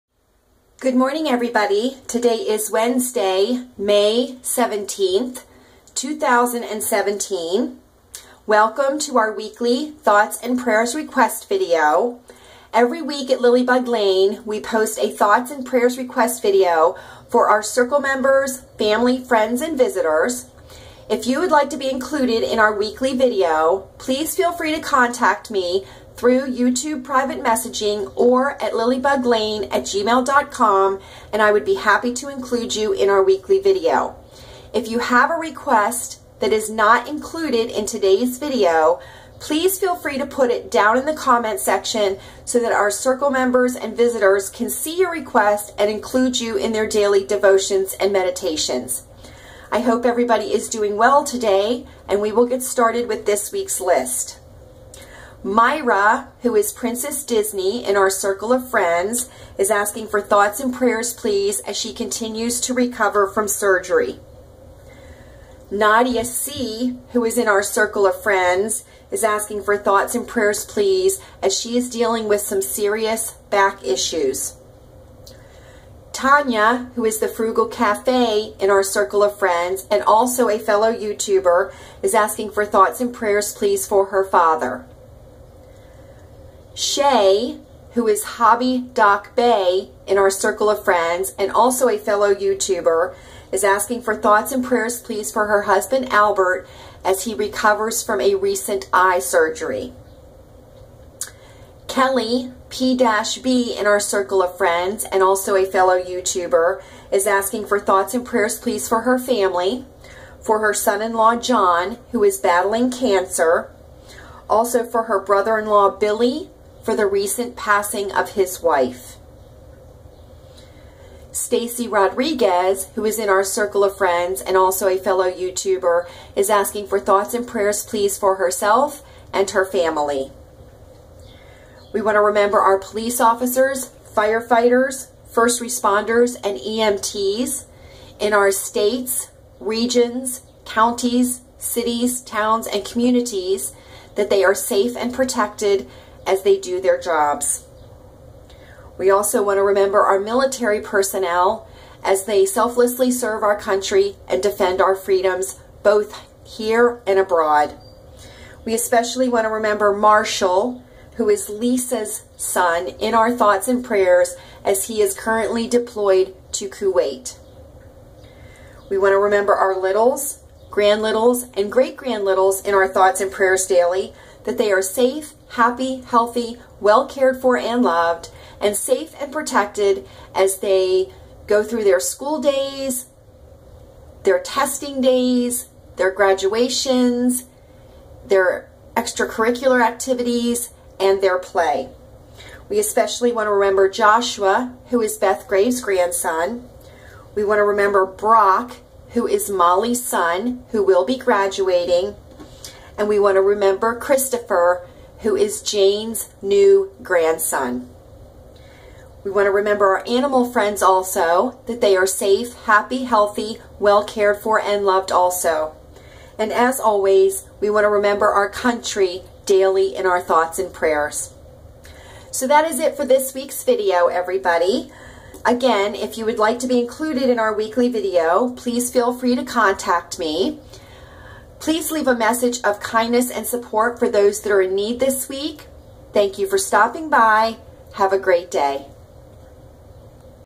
WEEKLY THOUGHTS and PRAYERS REQUESTS VIDEO... 5-17-17 Lilleybugglane
Welcome to our WEEKLY THOUGHTS and PRAYERS VIDEO... Please leave a message of KINDNESS and SUPPORT for those in need this week...If you have a request that is NOT included in this weeks video... Please leave it in the comment section... have a good day  xo lala